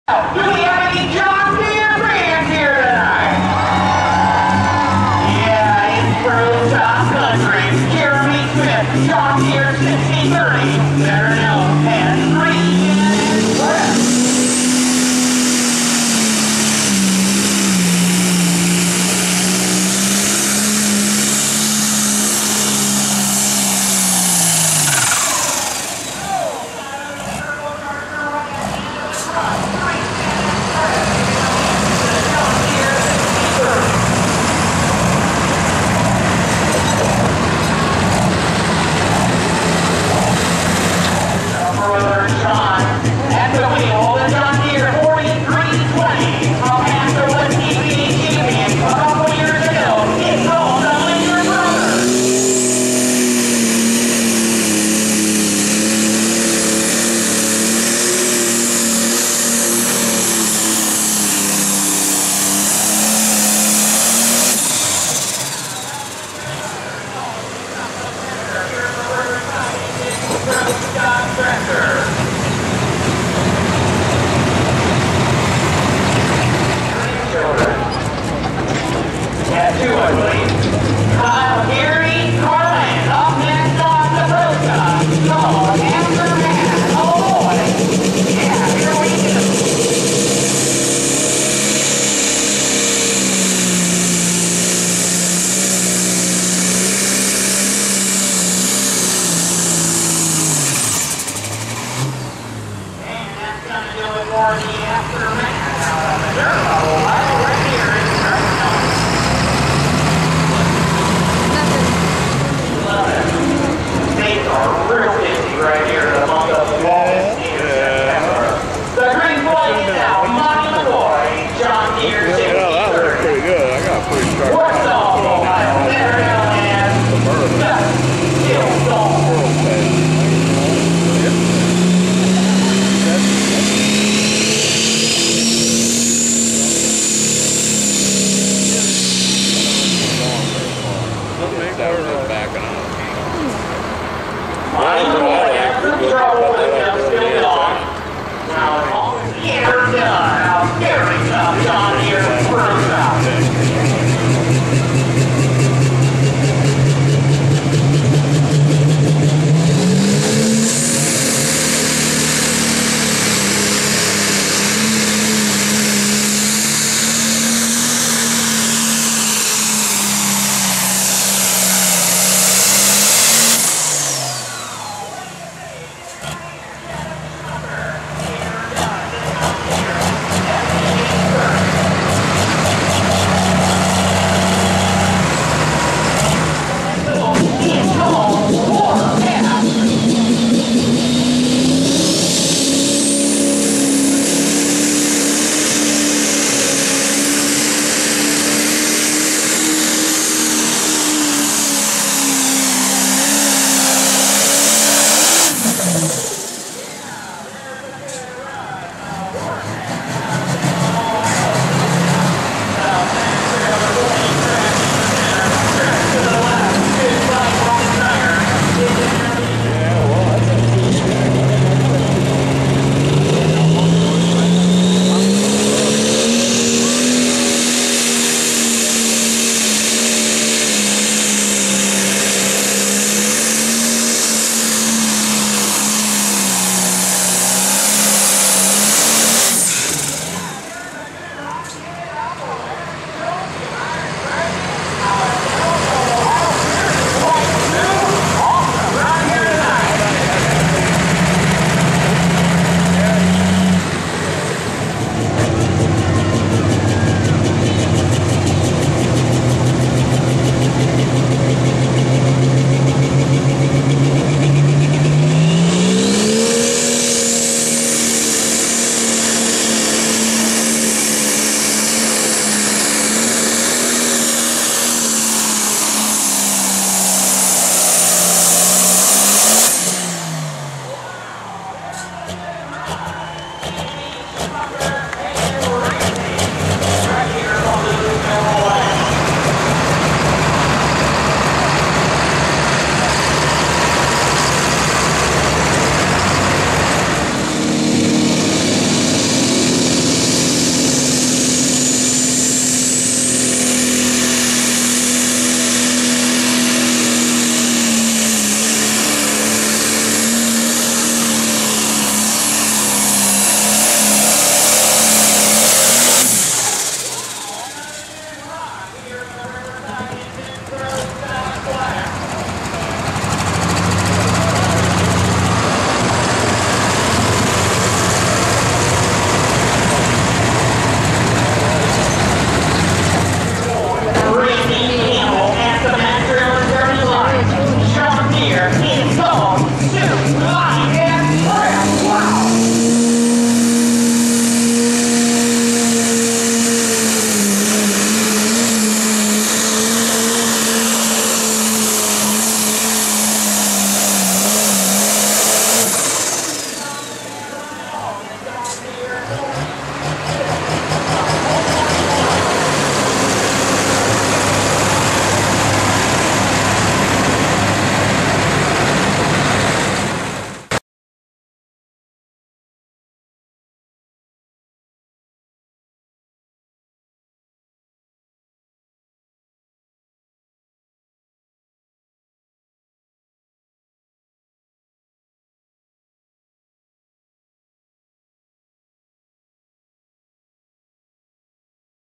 Pro Stocks Tractors pulling in Canton, OH - OSTPA 2011
Stark County Fair
Canton, OH
2011